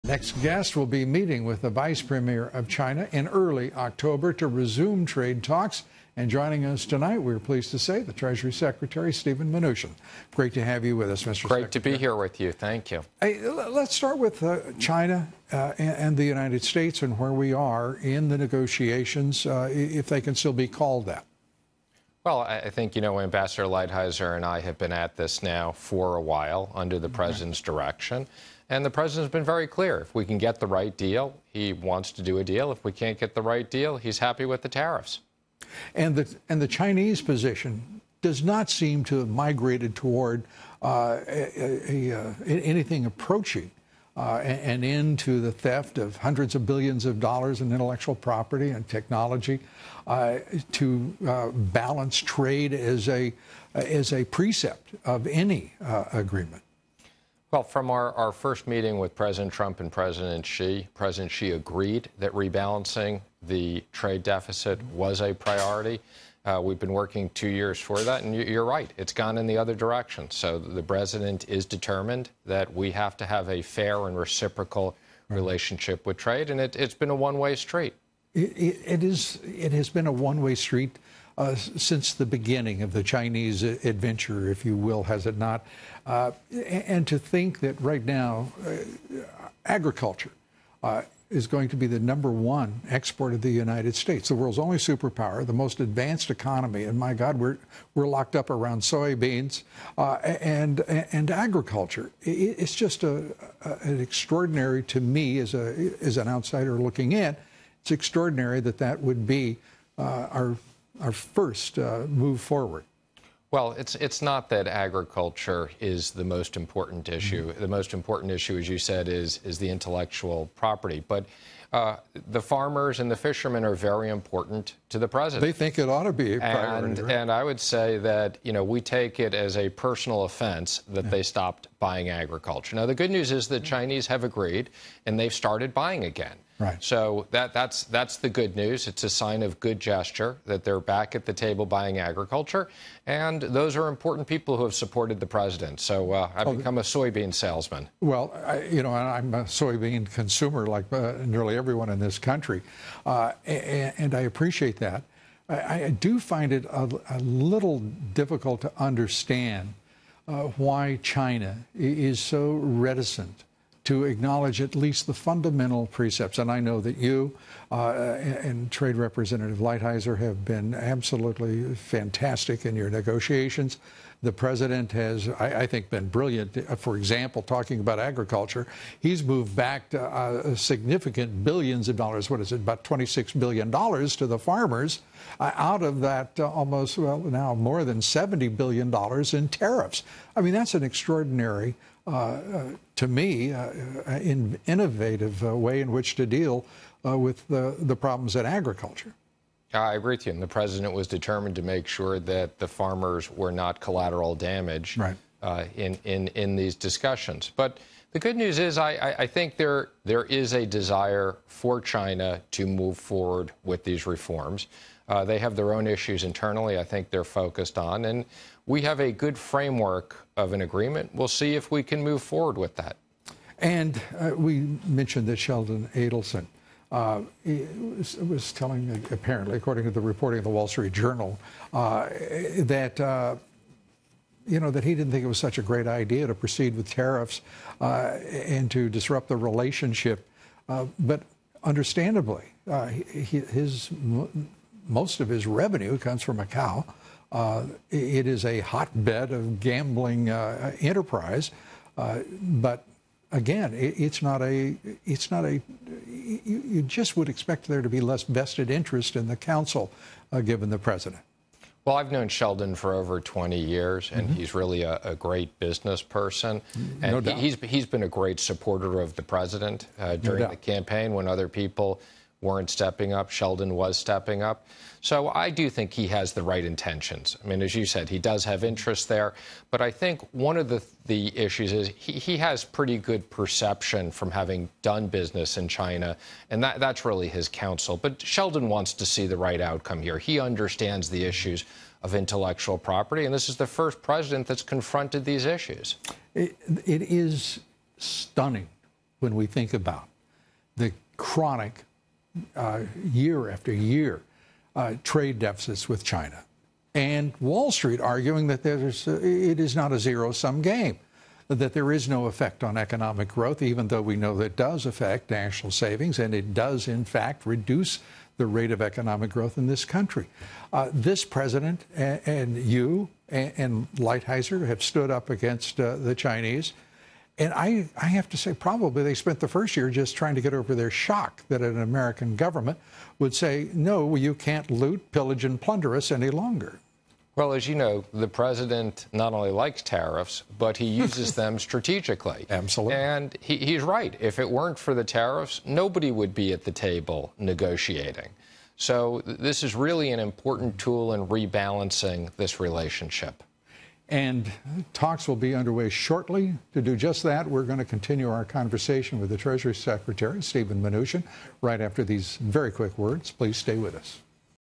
Mnuchin breaks down where US-China trade talks stand today
U.S. Secretary of the Treasury Steve Mnuchin talks to FOX Business' Lou Dobbs about the U.S.-China trade negotiations.